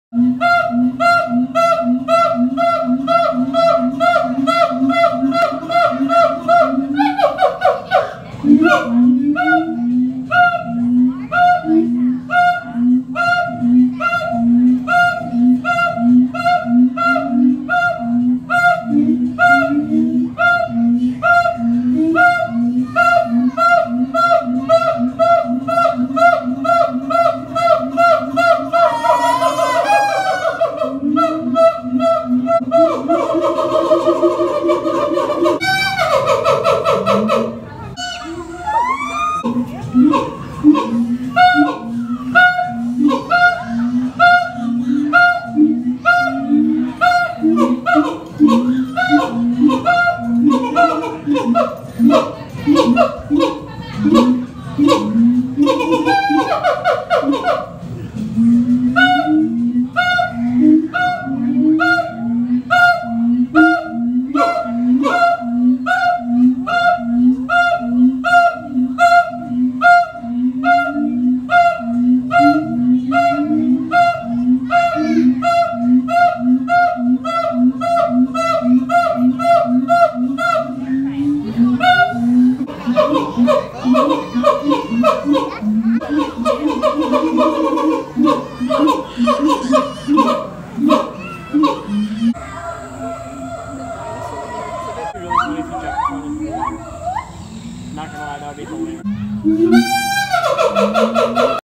Monkeys screaming at the zoo
i was at the zoo and these monkeys went bananas🤯